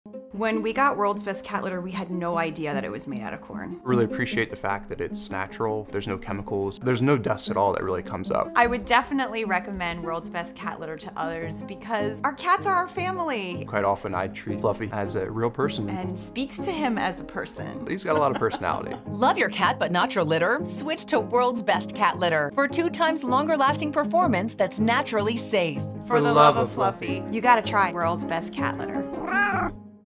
Cat Litter Made from Corn?
Is your cat like family? Give them the best with the only naturally safe litter made from whole-kernel corn.